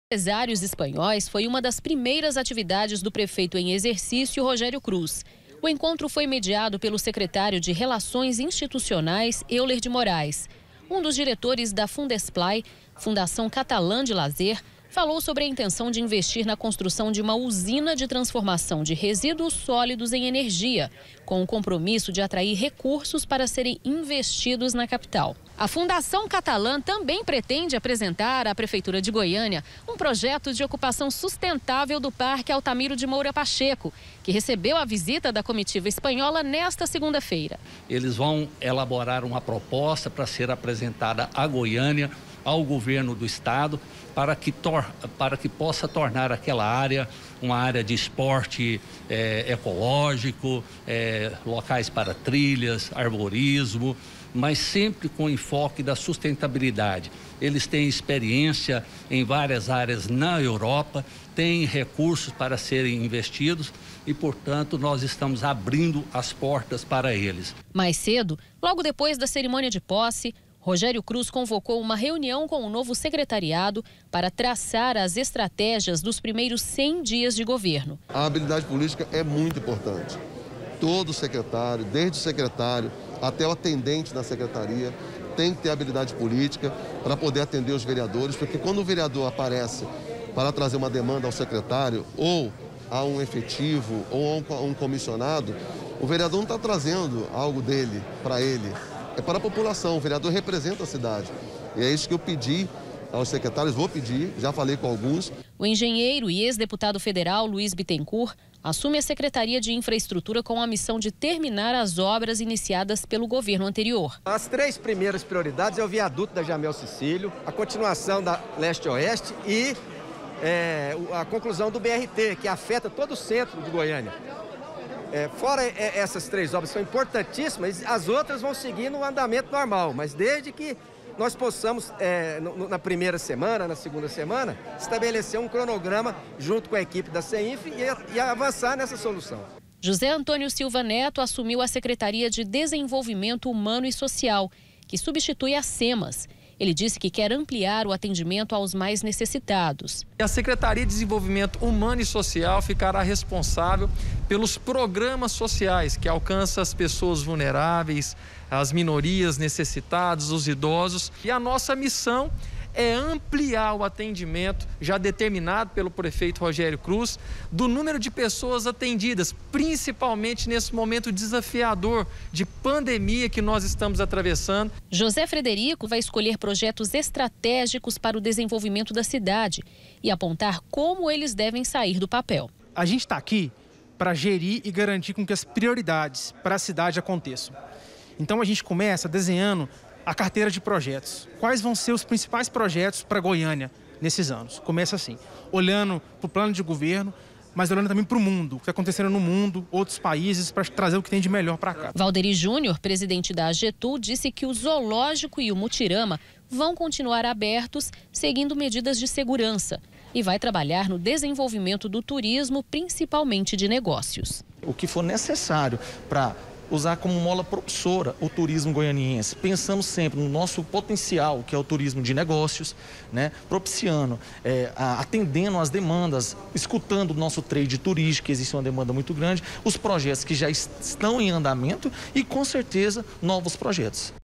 PREFEITURA DE GOIÂNIA: SECRETÁRIOS TOMAM POSSE E JÁ COMEÇAM A TRABALHAR
OS NOVOS SECRETÁRIOS DA PREFEITURA DE GOIÂNIA TOMARAM POSSE HOJE E JÁ COMEÇARAM A TRABALHAR. UMA NOVIDADE NA LISTA ANUNCIADA NO SÁBADO, É O SECRETÁRIO DE INFRAESTRUTURA, LUIZ BITTENCOURT, QUE SÓ FOI CONFIRMADO HOJE NO CARGO. ELE PROMETE PRIORIZAR A CONCLUSÃO DE OBRAS DEIXADAS PELO EX-PREFEITO IRIS REZENDE. NESSE PRIMEIRO DIA ÚTIL DA GESTÃO, O PREFEITO EM EXERCÍCIO ROGÉRIO CRUZ SE REUNIU COM OS AUXILIARES E RECEBEU INVESTIDORES ESPANHÓIS.